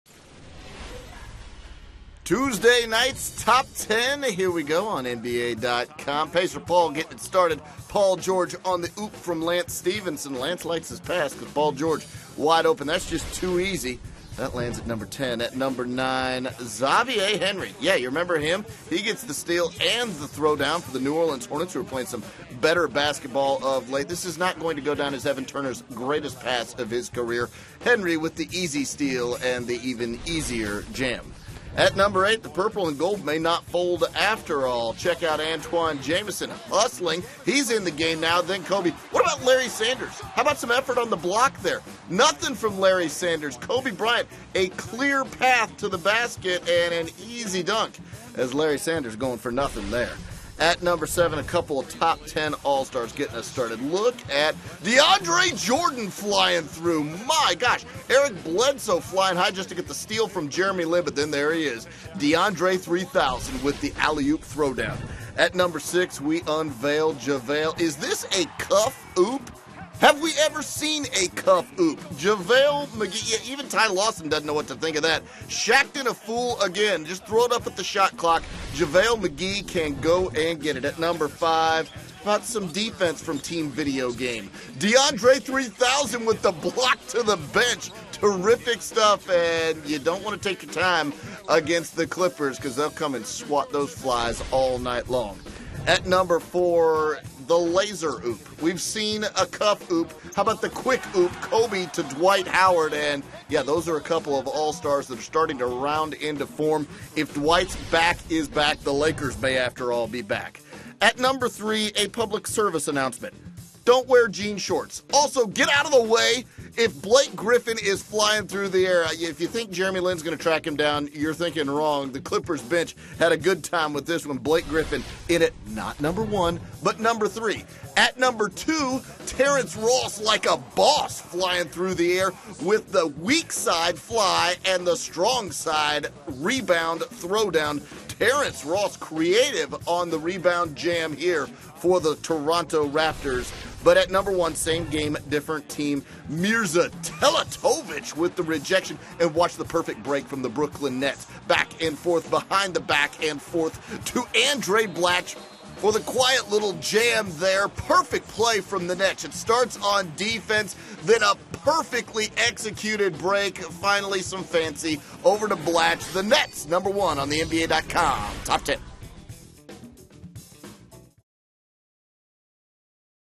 Tuesday's Top 10 | Clippers vs Rockets & More | Jan 15th, 2013
See what play from Tuesday's NBA action lands at the top spot of the Top 10.